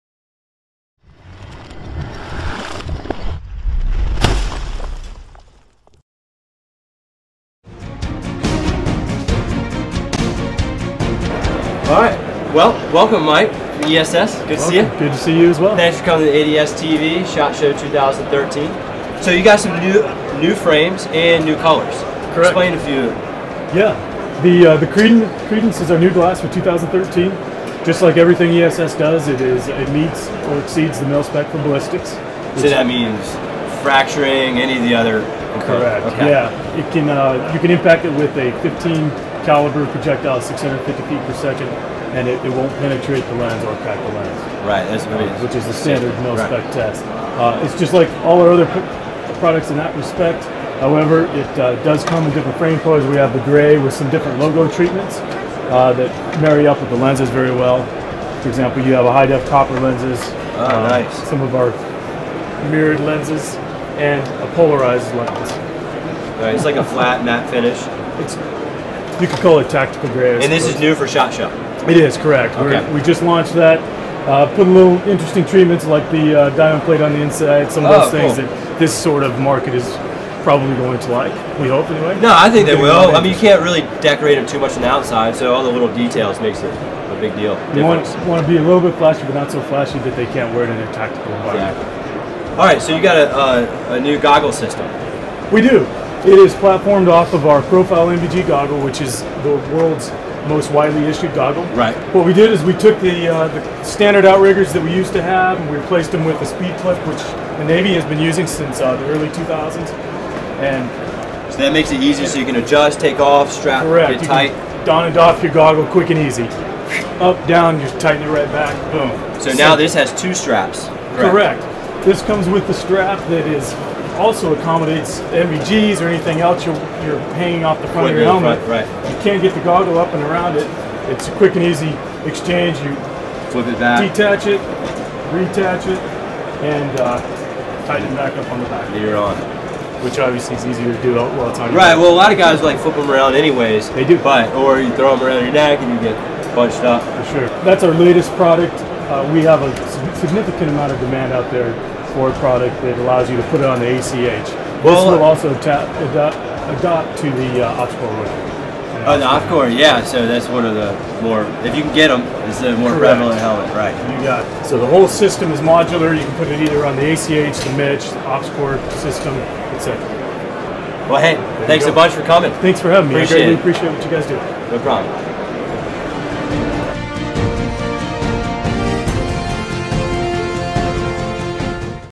2013 Shot Show Coverage | ADS, Inc. TV | ESS
Mike Vigueria from ESS introduces us their new Credence High-Impact Sunglasses and the new Profile series ballistic goggles!!